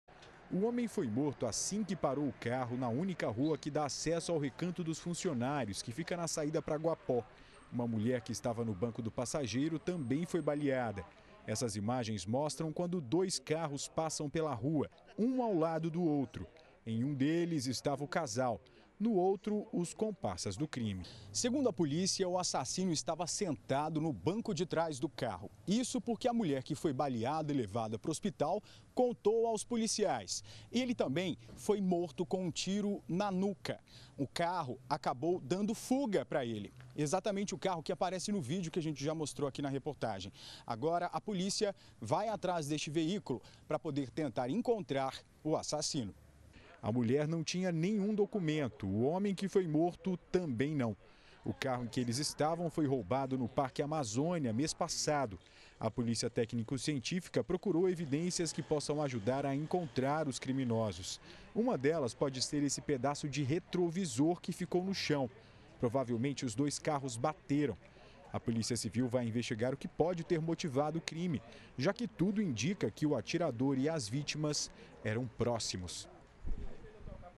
GA - Polícia investiga duplo homicídio - 17-08-2017
Imagens de circuito de segurança vão ajudar a polícia na investigação de  duplo homicídio no setor Recanto dos Funcionários. O carro em que as vítimas estavam tinha sido roubado no mês passado.